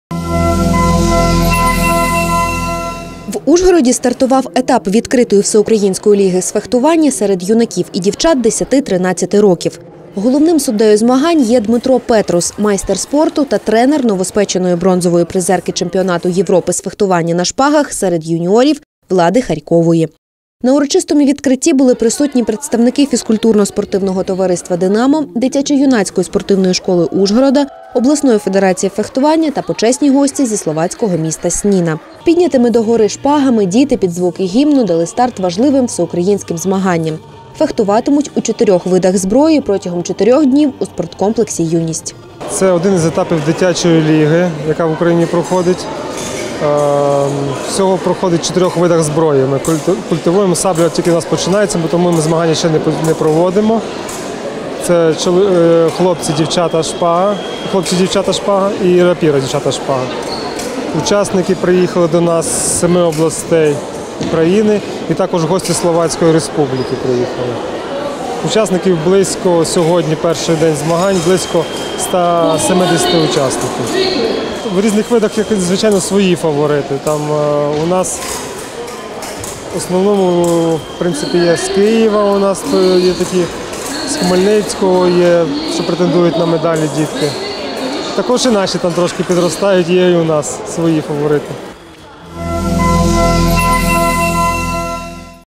Ужгород приймає етап Всеукраїнської дитячої ліги з фехтування
В Ужгороді стартував етап відкритої Всеукраїнської ліги з фехтування серед юнаків і дівчат 10-13 років. З піднятими догори шпагами діти під звуки гімну дали старт важливим Всеукраїнським змаганням. Фехтуватимуть у чотирьох видах зброї протягом 4 днів у спорткомплексі «Юність».